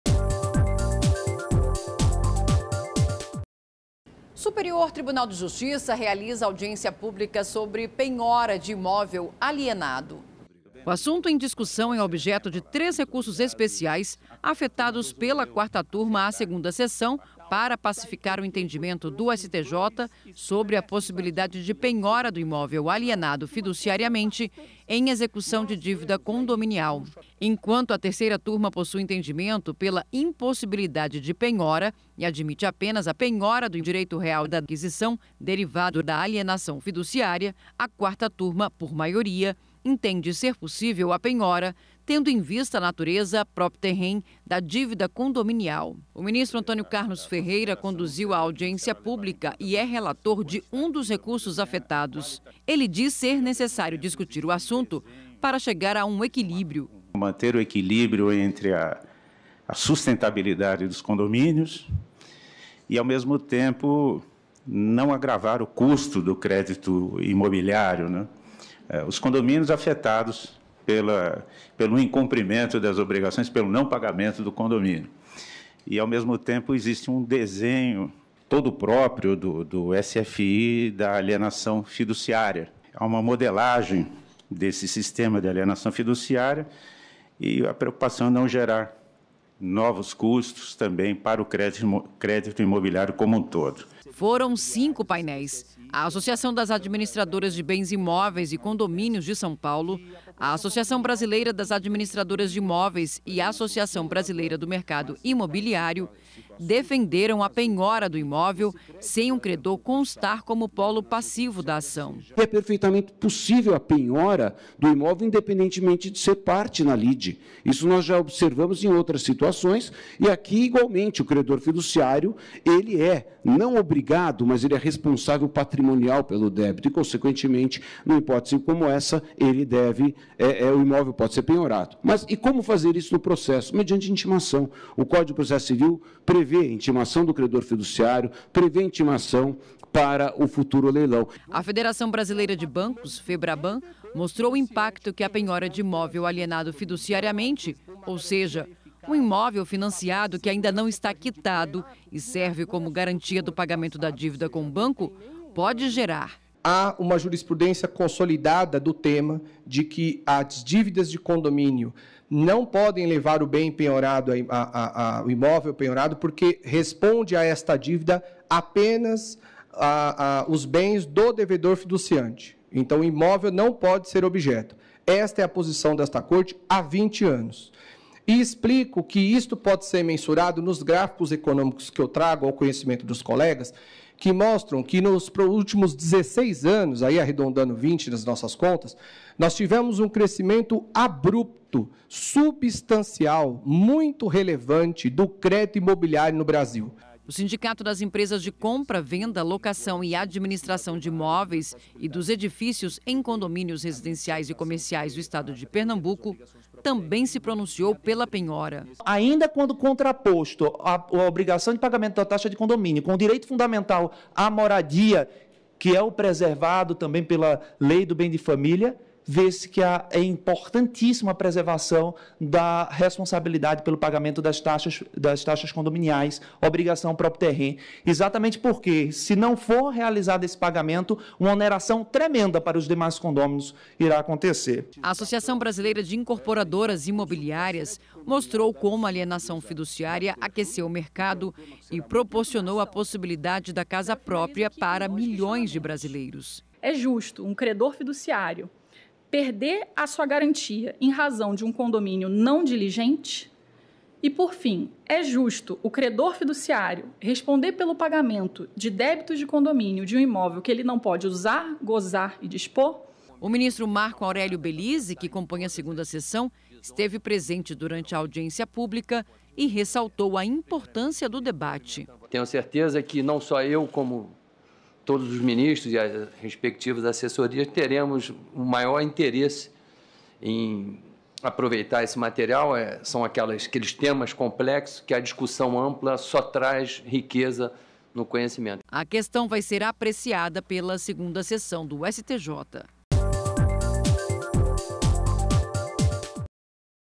Audiência discute penhora de imóvel alienado fiduciariamente para pagamento de dívida condominial
Durante a tarde desta segunda-feira (3), o Superior Tribunal de Justiça (STJ) reuniu especialistas para debaterem a possibilidade da penhora de imóvel com financiamento garantido por alienação fiduciária no curso da execução de débitos condominiais. O evento foi transmitido pelo canal do tribunal no YouTube.
O assunto é objeto de recursos especiais que, embora não tenham sido qualificados como repetitivos, foram afetados pela Quarta Turma à Segunda Seção para pacificação do tema no STJ.